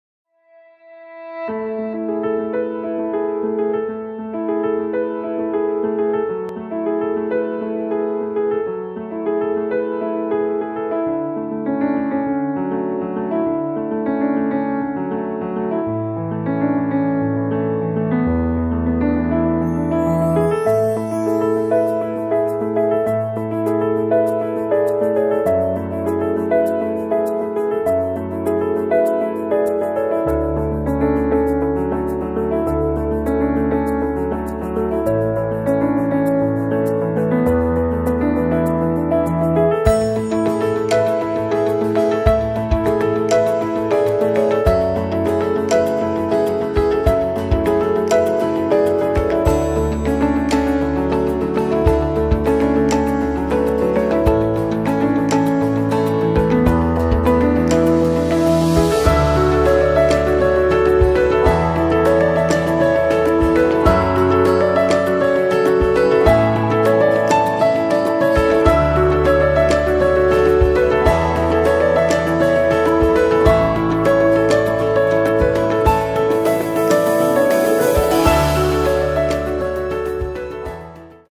فعالية نادي ذخر لكبار السن خلال اليوم العالمي لكبار السن
مقتطفات من زيارة سعادة خالد الكمده، وأعضاء نادي_ذخر_الاجتماعي لقرية البوم السياحية

Highlights from HE. Khaled Al Kamda & Thukher_Club members visit to Al Boom Tourist Village